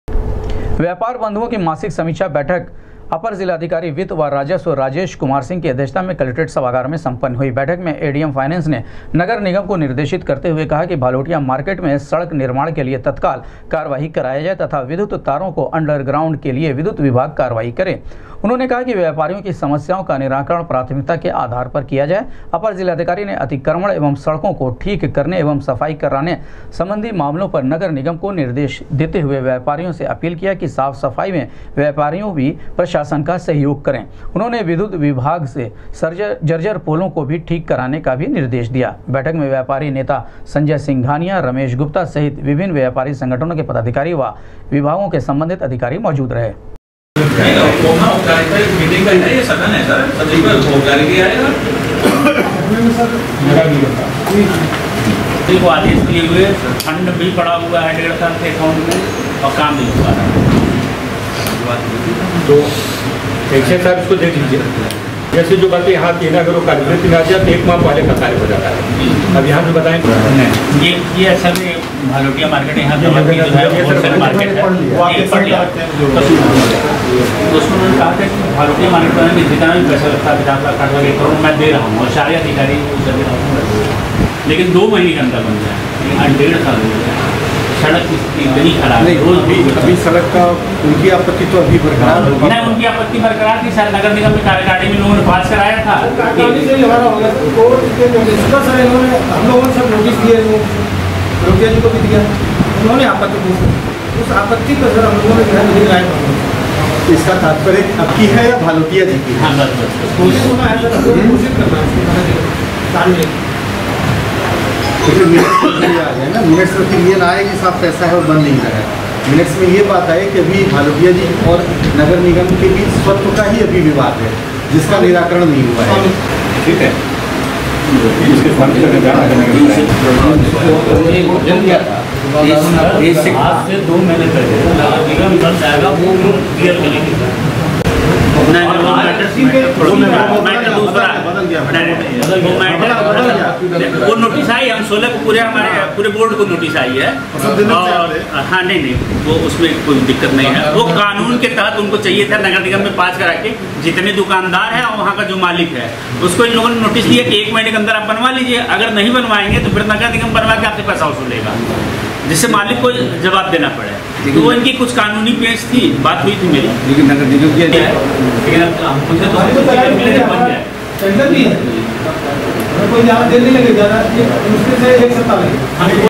एडीएम वित्त व राजस्व की अध्यक्षता में सम्पन्न हुई व्यापार बन्धुाओं की मासिक समीक्षा बैठक
एडीएम वित्त व राजस्व की अध्यक्षता में सम्पन्न हुई व्यापार बन्धुाओं की मासिक समीक्षा बैठक ,भालोटिया मार्केट की सड़कों के निर्माण सहित व्यापार बन्धुाओं के समस्याओं के त्वरित निस्तारण के दिये निर्देश